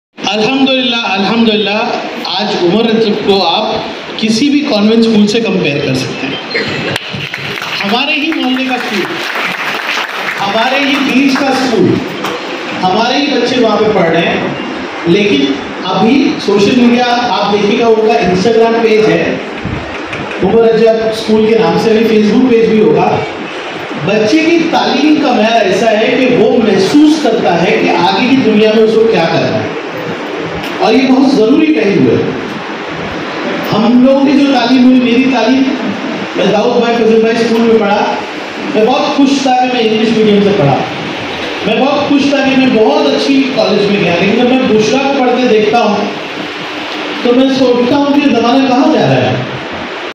MLA Rais Shaikh Distributed 188 TAB in a Career Guidance Programme at Madanpura
MLA RaisShaikh ne 188 Student ko TAB distribute Kiya

Madanpura Ansar Hall Me MLA Rais Shaikh Ne Apni NGO AHTESAB ki Janib Se Aaj Ek Career Guidance Program aur Educational Awareness Program Rakha Jisme Kafi Tadad Me Students Aur Parents Ki Shirqat Ki.

Iss Program me Students ki Councilling Ke liye Aamir Ansari (Principal Madani School),  Engineer Saeed Ahmed, Professor Athar Ansari aur Aisa Kafi Mahir Aur Kamyab Engineers aur Professor ne Bacho ki Rehnumayi ki.

Iske Awala Byculla Ilaqe se Numaya Kamyabi Hasil Kiye Gaye 25 Logo ko Felicitate Kiya Gaya.